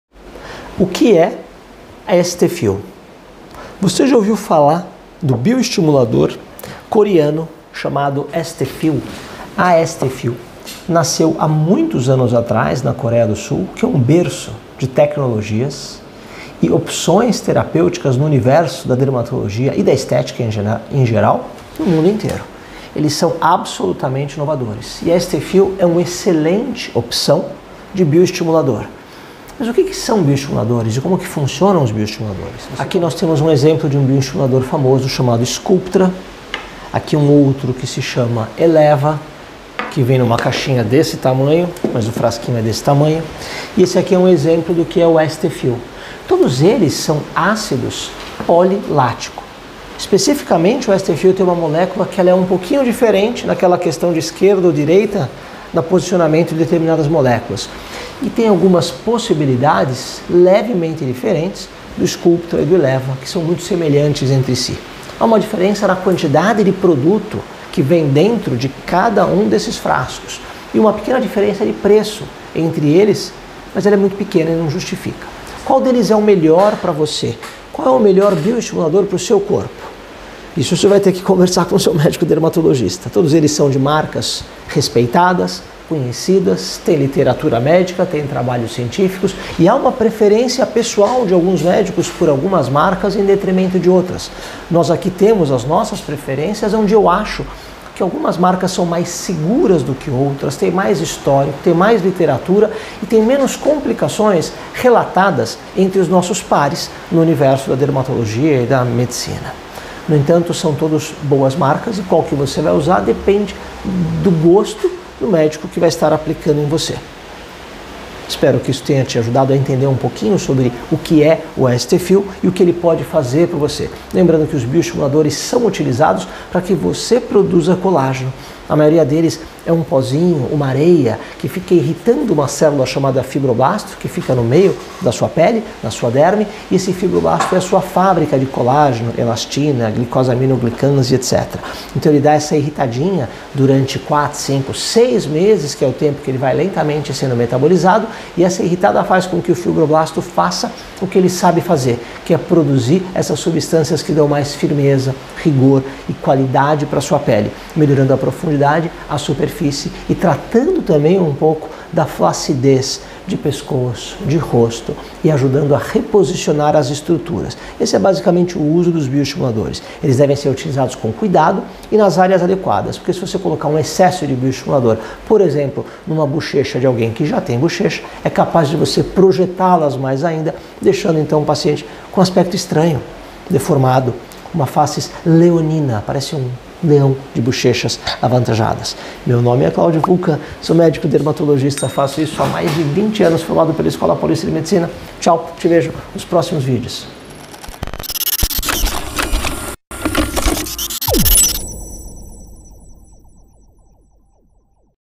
O que é Aesthefill?
É um poderoso estimulador de colágeno . É um preenchedor dérmico que causa regeneração tecidual profunda, a fim de reduzir os sinais de envelhecimento a longo prazo. Tem um incrível efeito antienvelhecimento, pois a capacidade de retenção de água do ácido polilático é 20 vezes maior que a do ácido hialurônico, sendo capaz de restaurar as rugas em um nível mais profundo.